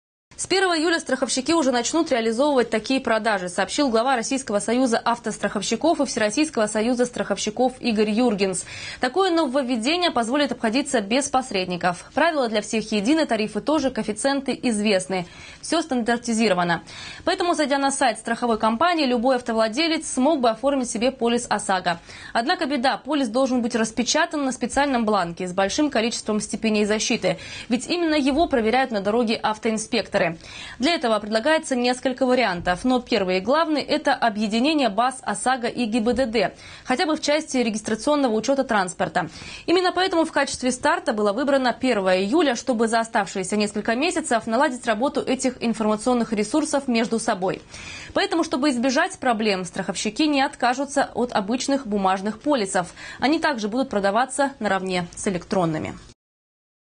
С 1 июля полис ОСАГО можно будет купить в электронном виде. Новости 24 Сочи Эфкате
Оформить его и оплатить можно будет через Интернет. А потом просто распечатать на принтере. Об этом сообщает сегодня «Российская газета».
Сочи. Новости.
Телеканал Эфкате.